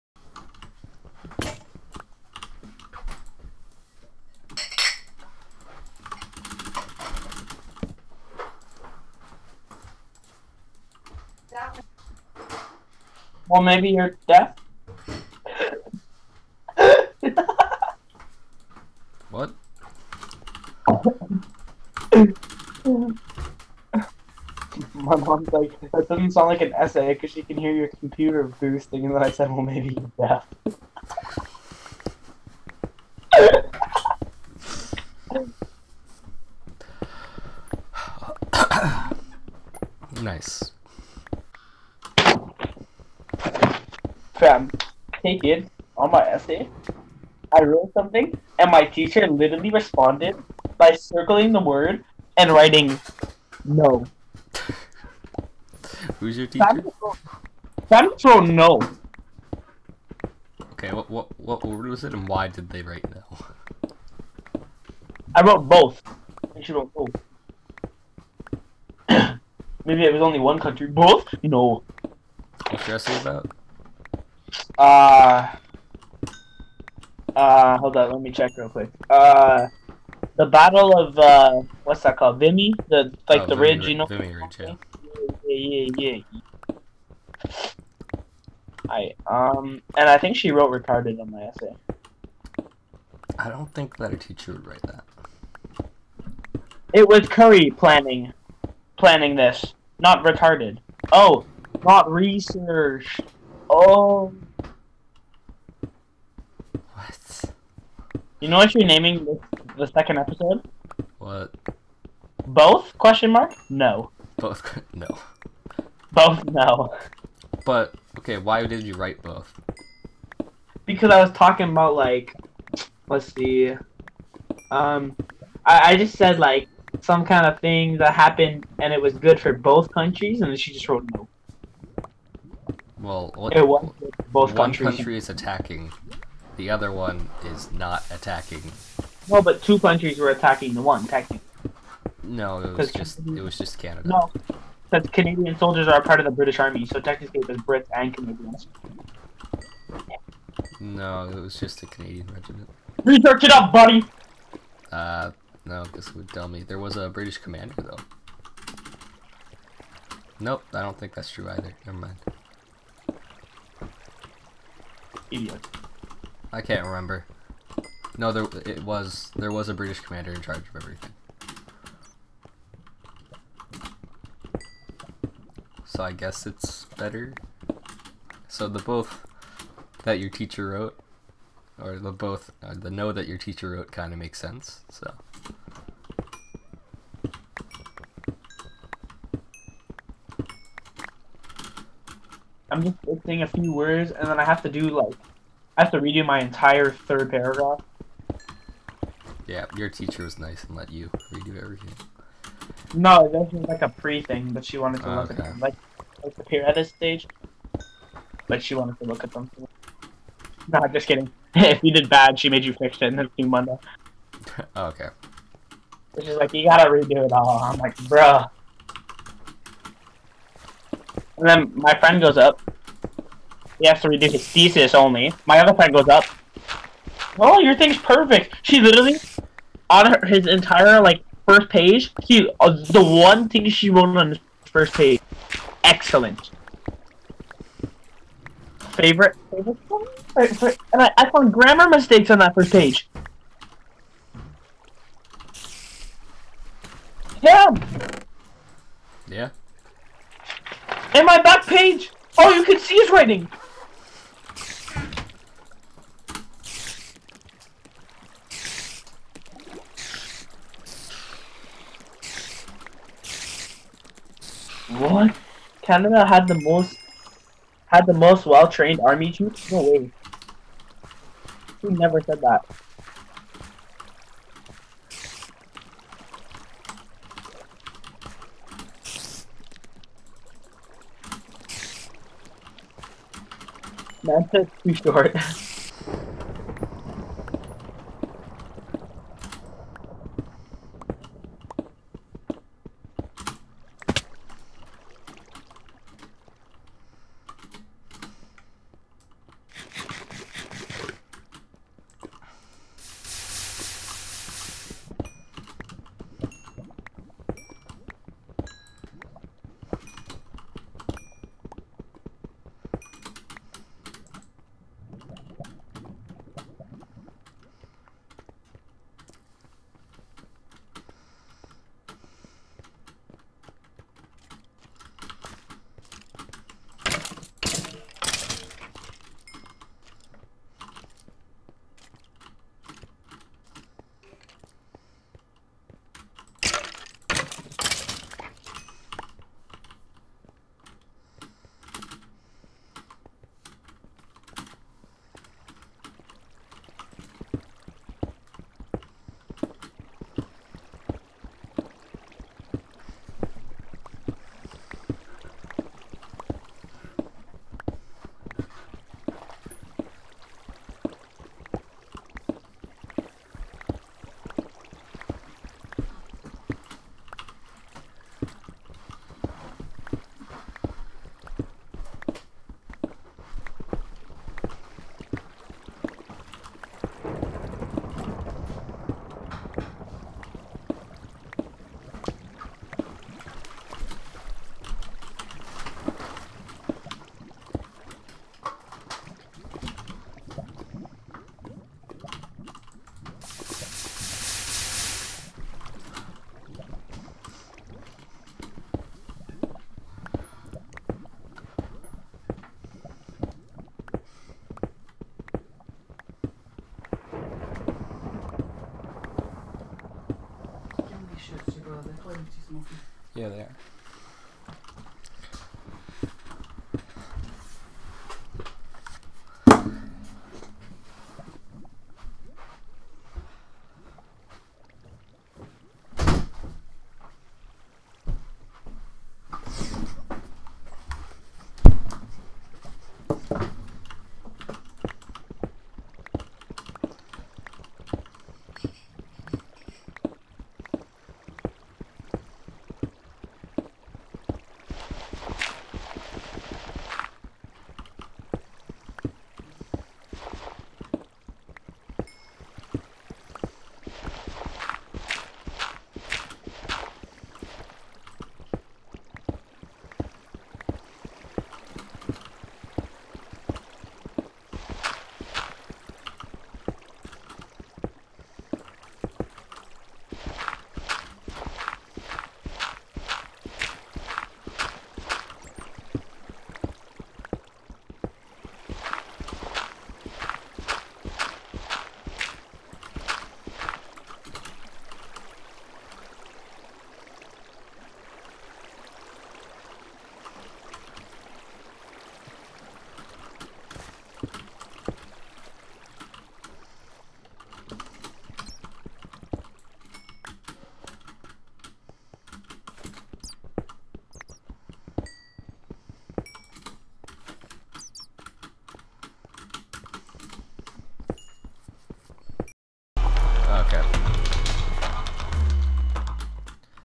Playground Uhc Season 2 EP 2 - Still Mining
Welcome to a recorded round of Ultra Hardcore organized by Tay. The second season of Playground UHC. The themes for this round consists of FFA, timebomb, and cutclean. This season of Playground UHC is hosted by Kingdom. Players: 3Tips Exponic Evzen FreekBoi Arslab Stravilight Boltz Ough Mknight Luka Gaming LunarWinterWolf Tay Taygran Zyaf TrueFour